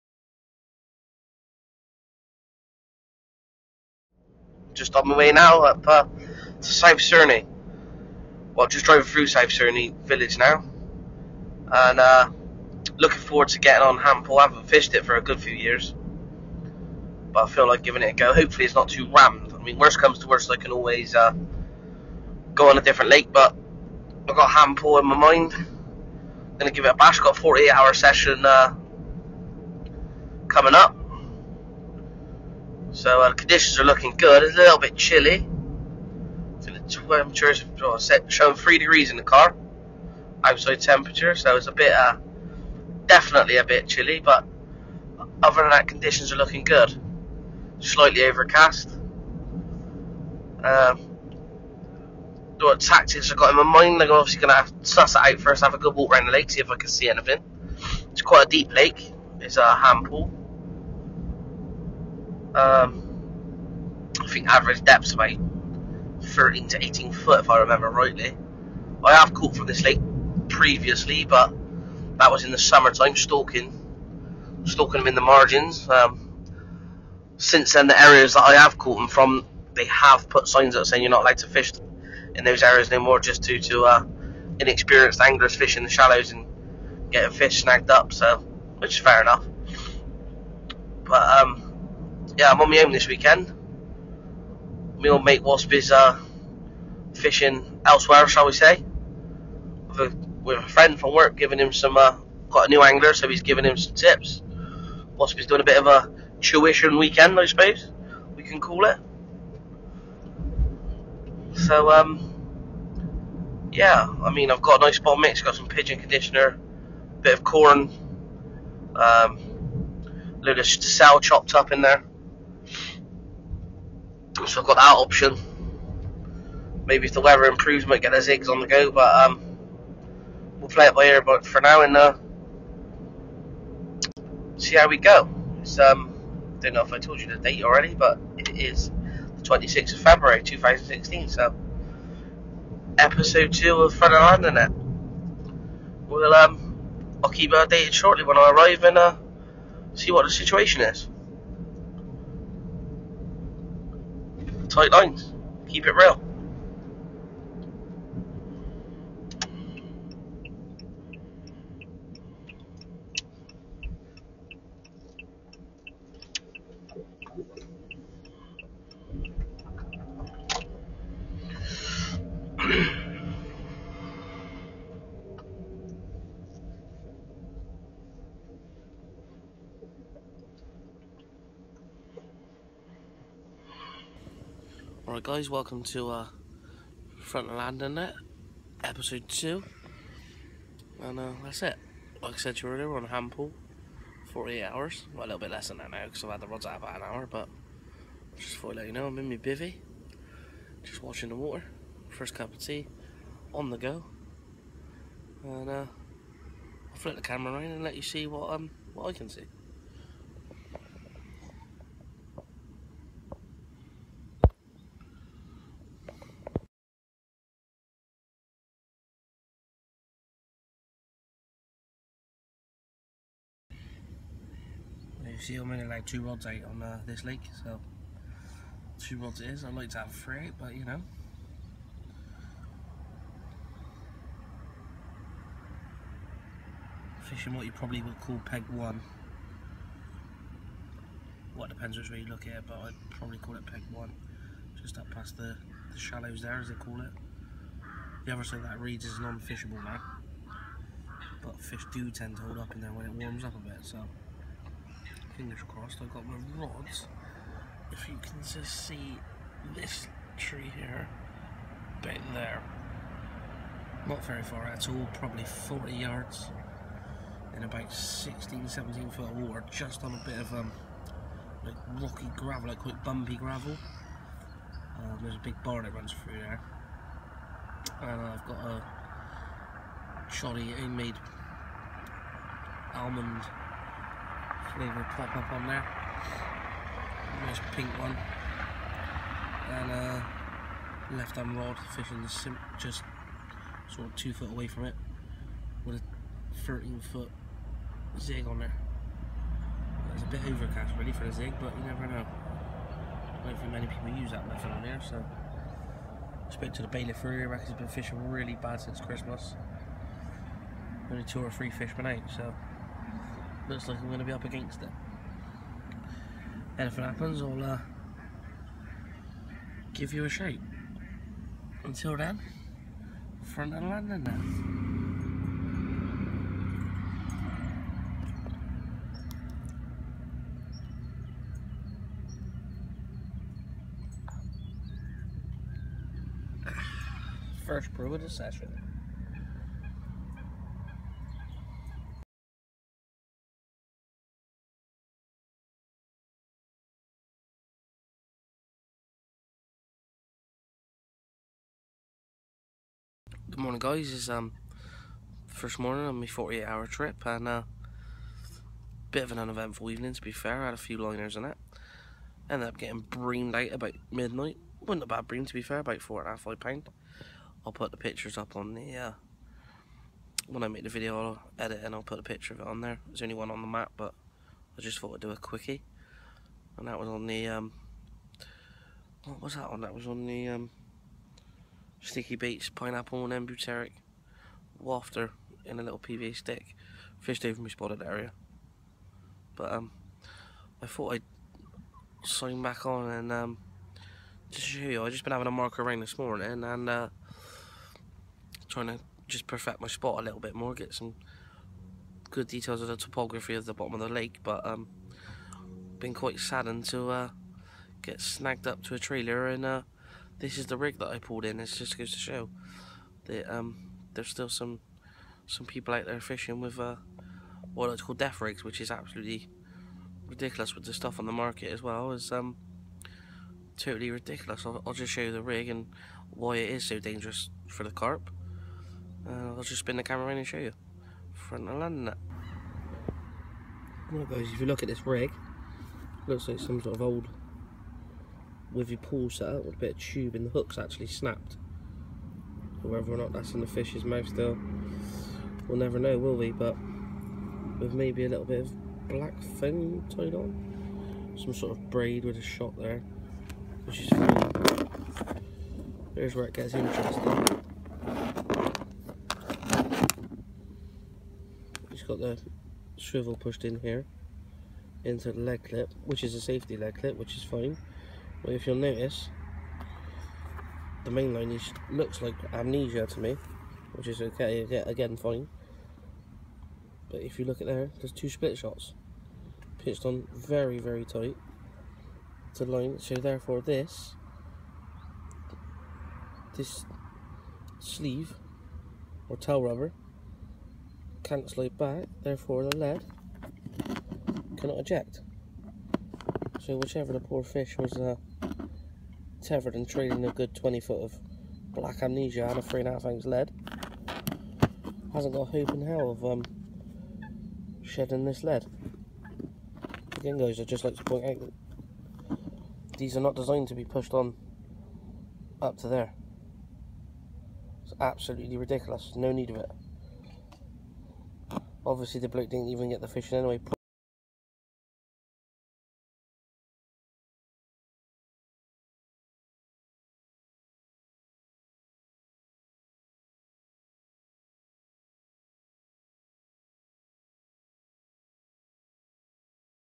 Ham pool south Cerney Front of The Landing net ep2
48hr carp fishing session ham pool south Cerney